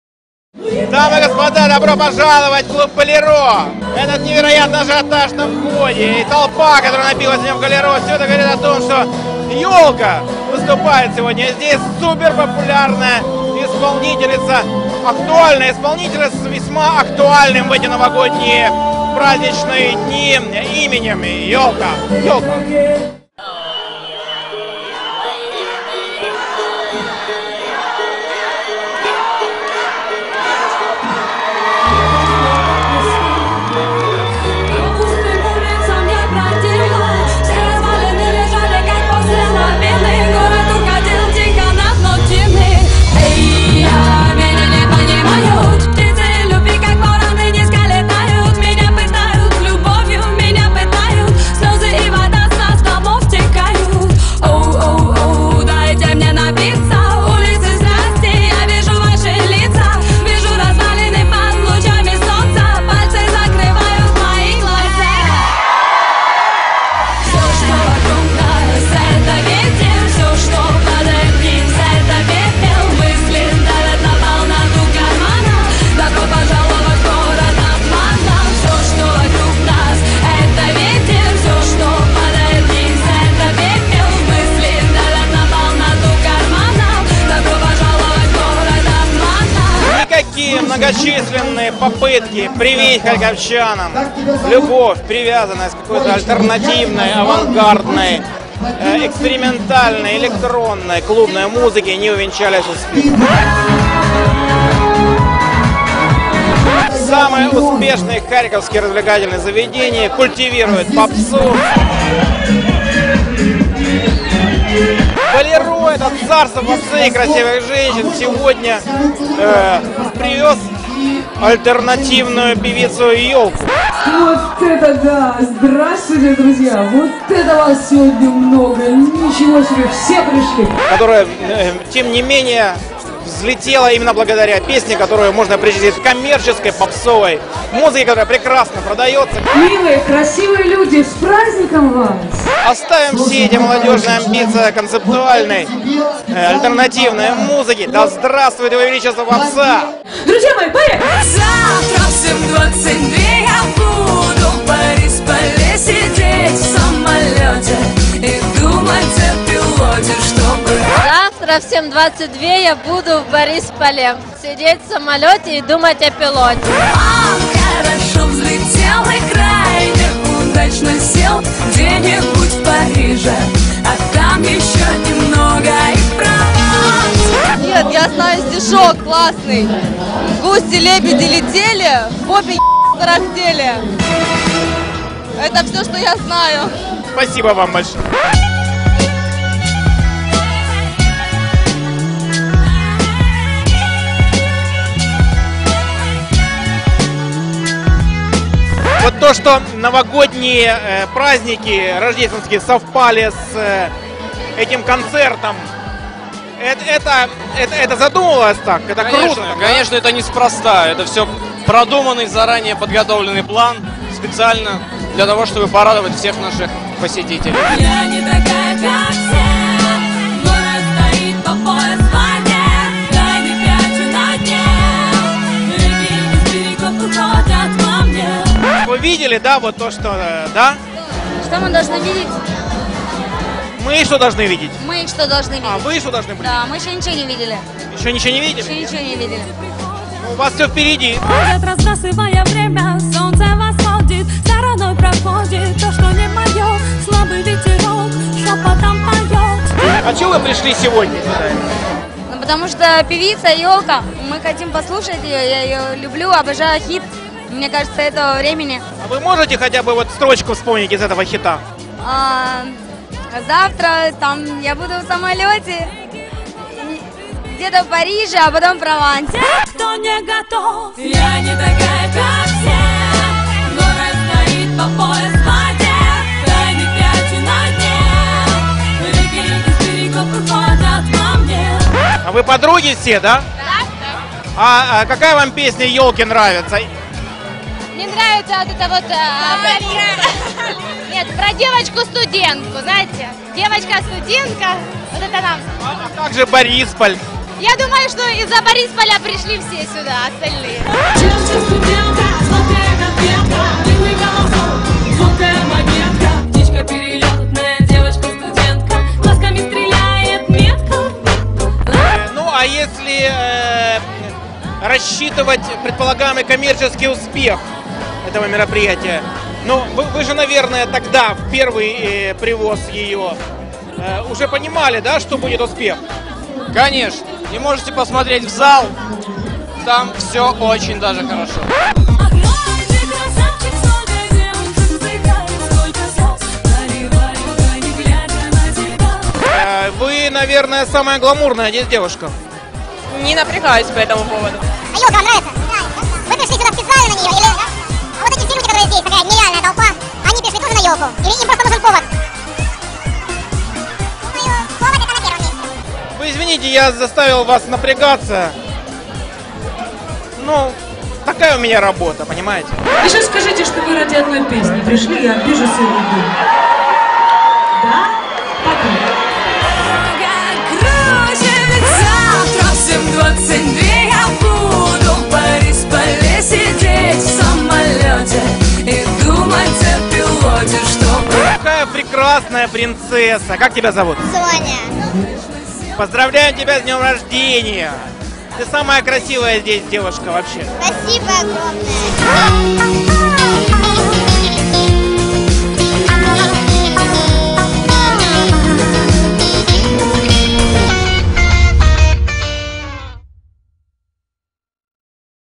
Концерт Ёлки. Харьков, клуб Bolero, 7 января 2011
Видеооператор - Денис Агеенко
Монтаж - креатив production